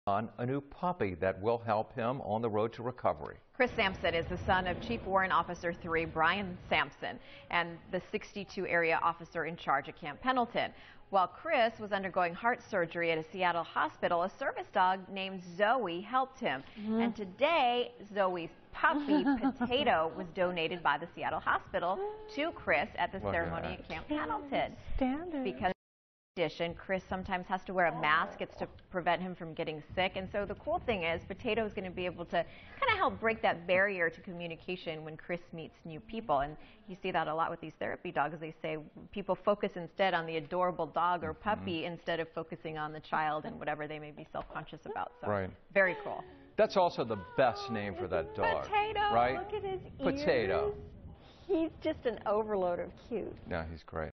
Camp Pendleton Marine's Son Receives Service Dog - NBC7
NBC7 covers Chris Simpson receiving his service dog, Potato. James and Cat Skoor donated the puppy to the Simpson family during a short ceremony aboard Camp Pendleton. The Skoor family met Chris while he was recovering from heart transplant surgery in Washington. Their Dog Zoe comforted Chris in the hospital over a year ago. Since that time, Zoe had puppies, one of which was Potato.